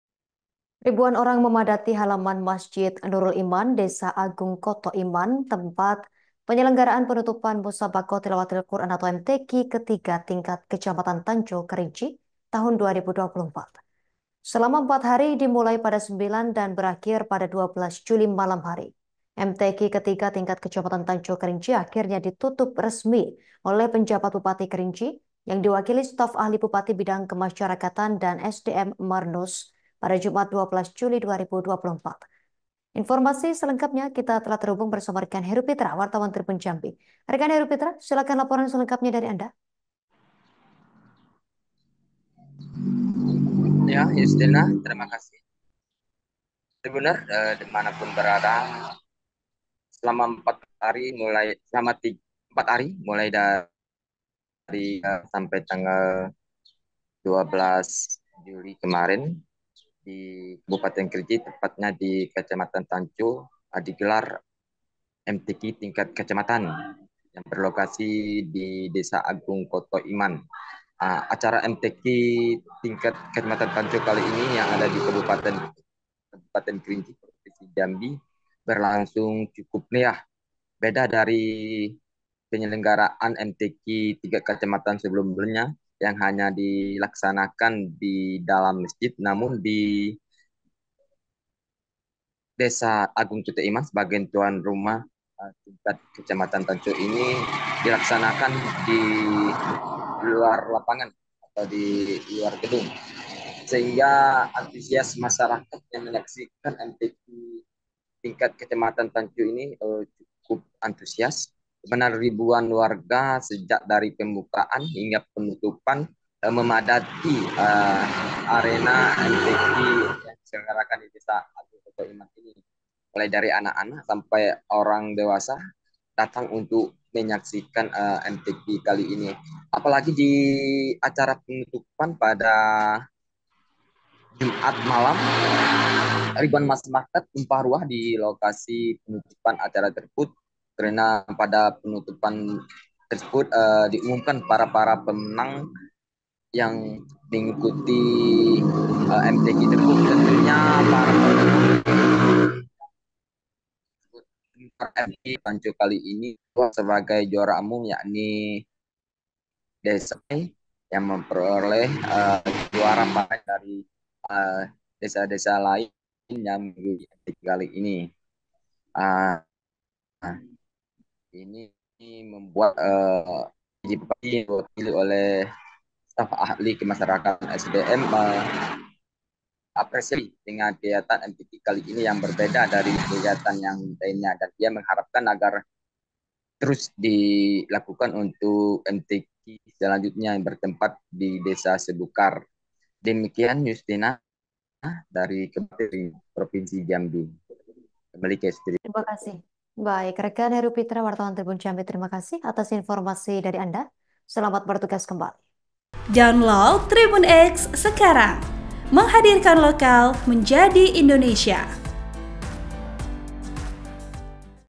Penutupan MTQ di Tanco Diikuti Ribuan Orang, Marnus Sampaikan Apresiasi untuk Seluruh Kafilah
Laporan wartawan Tribun Jambi - Herupitra
TRIBUN-VIDEO.COM-Ribuan orang memadati halaman Masjid Nurul Iman Desa Agung Koto Iman tempat penyelenggaraan penutupan Musabaqoh Tilawatil Qur’an (MTQ) ke 3 tingkat Kecamatan Tanco, Kerinci Tahun 2024.

Selama empat hari dimulai pada 9  dan berakhir 12 Juli malam, MTQ ke 3 tingkat Kecamatan Tanco, Kerinci akhirnya ditutup resmi oleh Pj Bupati Kerinci yang diwakili Staf Ahli Bupati Bidang Kemasyarakatan dan SDM, Marnus pada, Jumat (12/7/2024) malam.(*)

Program: Live Update
Host: Yustina Kartika Gati
Editor Video: Ika Vidya Lestari
Uploader: Restu Riyawan